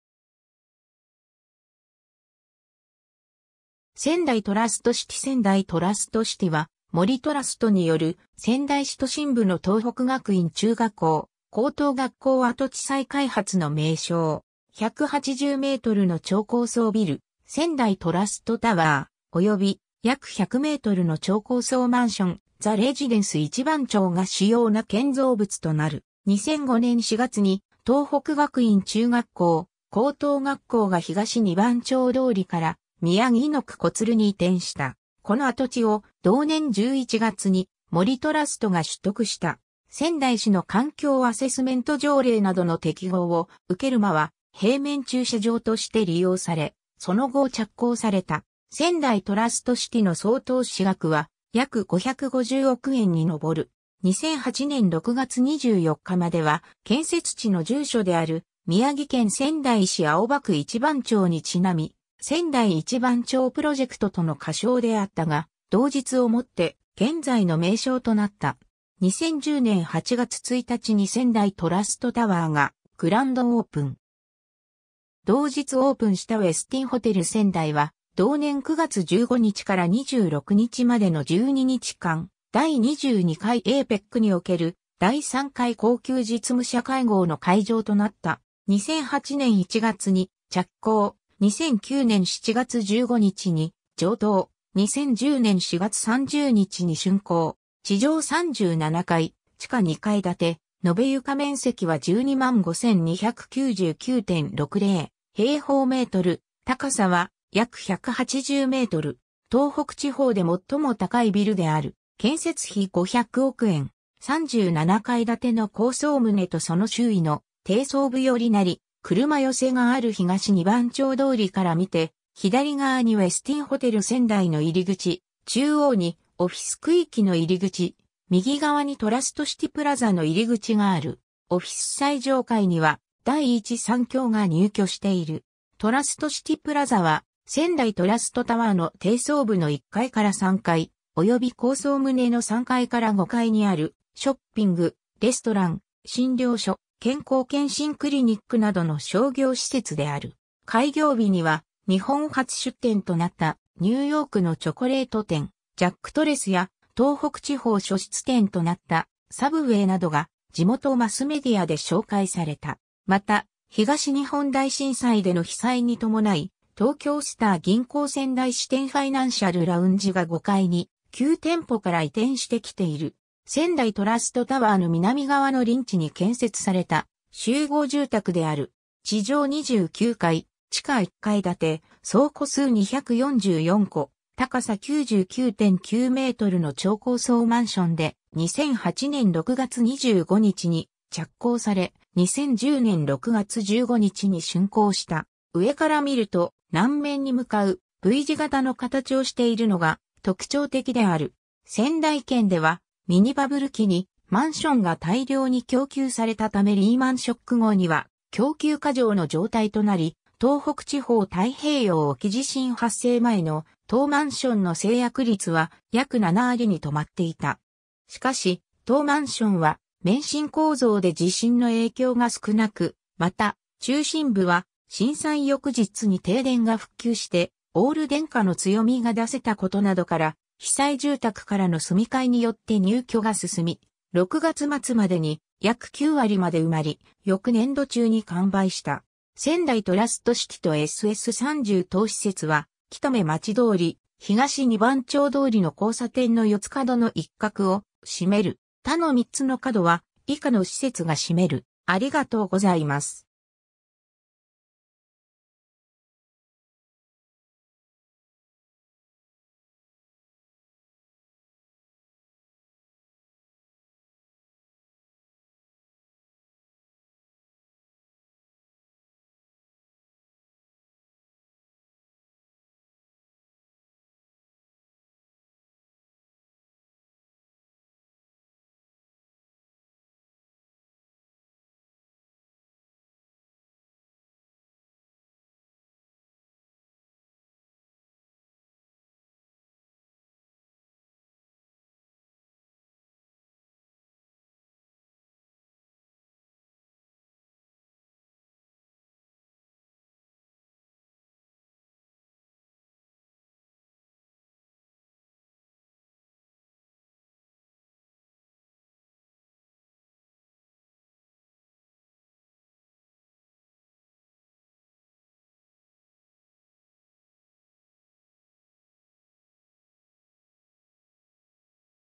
仙台トラストシティ
仙台トラストシティ（2010年6月1日） 仙台トラストシティ（せんだいトラストシティ）は、森トラストによる仙台市都心部の東北学院中学校・高等学校跡地再開発の名称。
 180mの超高層ビル「仙台トラストタワー」、および、約100mの超高層マンション「ザ・レジデンス一番町」が主要な建造物となる。
 2005年（平成17年）4月に東北学院中学校・高等学校が東二番丁通りから宮城野区小鶴に移転した。
この跡地を同年11月に森トラストが取得した（取得額は不明）。
仙台市の環境アセスメント条例などの適合を受ける間は平面駐車場として利用され、その後着工された。
仙台トラストシティの総投資額は約550億円にのぼる。
 2008年（平成20年）6月24日までは、建設地の住所である宮城県仙台市青葉区一番町に因み、仙台一番町プロジェクト（せんだいいちばんちょうプロジェクト）との仮称であったが、同日を以って現在の名称となった。
 2010年（平成22年）8月1日に仙台トラストタワーがグランドオープン。
同日オープンしたウェスティンホテル仙台は、同年9月15日 - 26日までの12日間、第22回APECにおける第3回高級実務者 (SOM3) 会合の会場となった。
 2008年（平成20年）1月に着工、2009年（平成21年）7月15日に上棟、2010年（平成22年）4月30日に竣工。
地上37階・地下2階建て、延べ床面積は12万5299.60m2（約3万7900坪）、高さは約180m。
東北地方で最も高いビルである。
建設費500億円。
 37階建ての高層棟とその周囲の低層部よりなり、車寄せがある東二番丁通りから見て左側（南側）にウェスティンホテル仙台の入口、中央にオフィス区域（6階〜24階）の入口、右側（北側）にトラストシティプラザの入口がある。
オフィス最上階には第一三共（株）が入居している。
 トラストシティプラザ（英称：TRUST CITY PLAZA）は、仙台トラストタワーの低層部の1階～3階および高層棟の3階～5階にあるショッピング、レストラン、診療所、健康検診クリニック(企業経由の予約制が原則で、個人は、3Fの東二番町診療所 に受診することで一部の設備を活用する形で行う)などの商業施設である。
 開業日には、日本初出店となったニューヨークのチョコレート店「ジャック・トレス」や、東北地方初出店となった「サブウェイ」などが地元マスメディアで紹介された。
また東日本大震災での被災に伴い、東京スター銀行仙台支店ファイナンシャル・ラウンジが5Fに旧店舗から移転してきている。
 仙台トラストタワーの南側の隣地に建設された集合住宅である。
地上29階・地下1階建て、総戸数244戸（分譲125戸、賃貸119戸）、高さ99.9mの超高層マンションで、2008年（平成20年）6月25日に着工され、2010年（平成22年）6月15日に竣工した。
上から見ると、南面に向かうV字型の形をしているのが特徴的である。
 仙台圏ではミニバブル期にマンションが大量に供給されたためリーマンショック後には供給過剰の状態となり、東北地方太平洋沖地震（東日本大震災）発生前の当マンションの成約率は約7割に留まっていた。
しかし、当マンションは免震構造で地震の影響が少なく、また、中心部は震災翌日に停電が復旧してオール電化の強みが出せたことなどから、被災住宅からの住み替えによって入居が進み、6月末までに約9割まで埋まり、翌年度中に完売した。
 仙台トラストシティとSS30（AERから撮影） 当施設は、北目町通り・東二番丁通りの交差点の四つ角の一角を占める。
他の3つの角は以下の施設が占める。